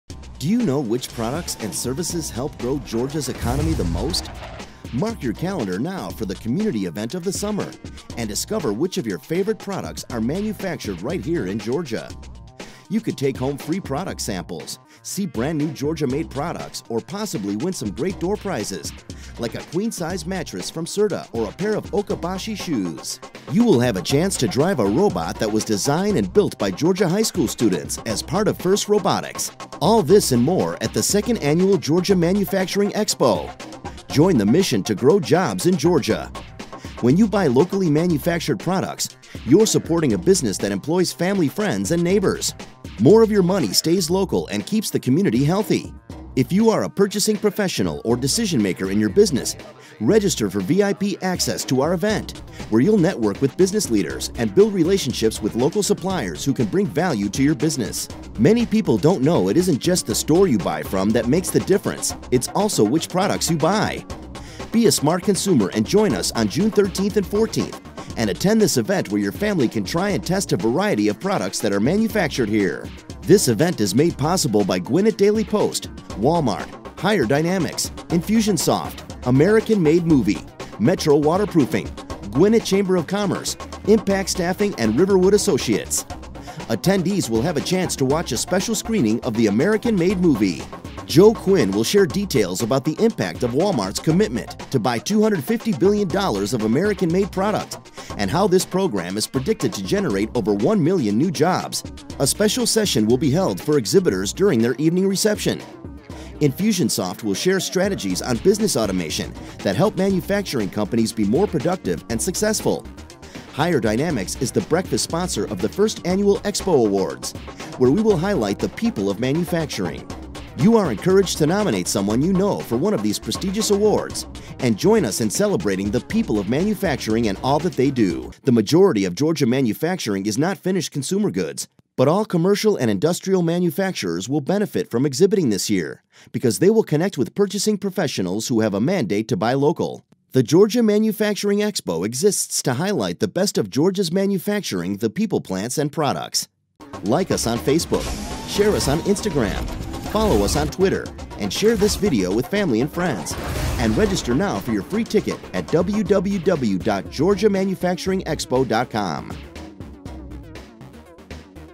2014 Georgia Manufacturing Expo Promo
June 13th & 14th -- Gwinnett Center --Duluth, GA

• Lt. Gov. Casey Cagle, who will be presenting the EXPO Awards to the People that make manufacturing happen in Georgia
• Wal-Mart, who has sent one of their executives (Joe Quinn) to the EXPO to share information on the organization's $250B project to invest in U.S. Manufacturing
• Southwire, the 2013 Georgia Manufacturer of the Year, who has one of their senior leadership team members (Mike Wiggins) presenting their 12 For Life program
• Hire Dynamics, the largest Georgia-based staffing company has chosen to become the title sponsor for the Georgia Manufacturing EXPO Awards
• "American Made Movie", an award-winning documentary that shares the story of U.S. Manufacturing, will be giving a free viewing during the EXPO